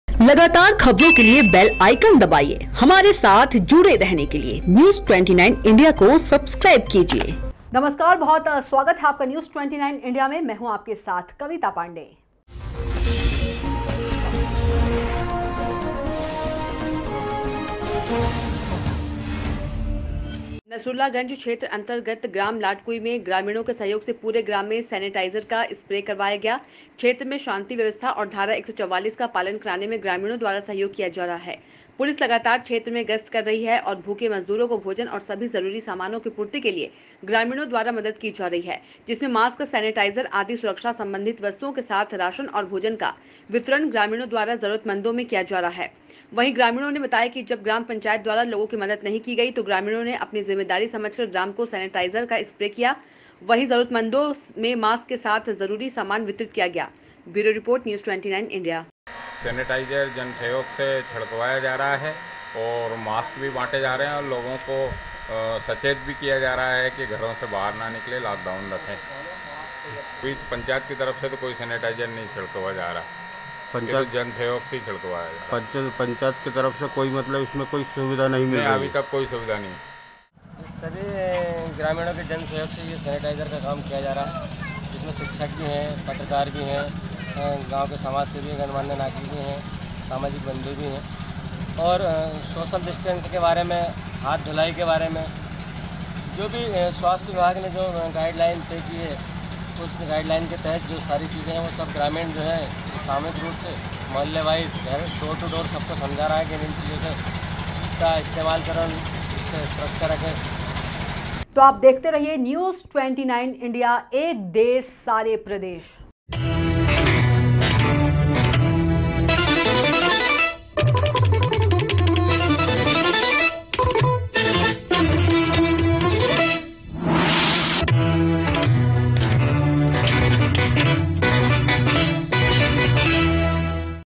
2_April News29India#New 7#ग्रामीणों के सहयोग से ग्राम में किया सेनेटाइजर का छिड़काव
ग्रामीणों के सहयोग से ग्राम में किया सेनेटाइजर का छिड़काव
ग्रामीणों मे किया मास्क का वितरण, कहा करें शासन के निर्देश का पालन#News29india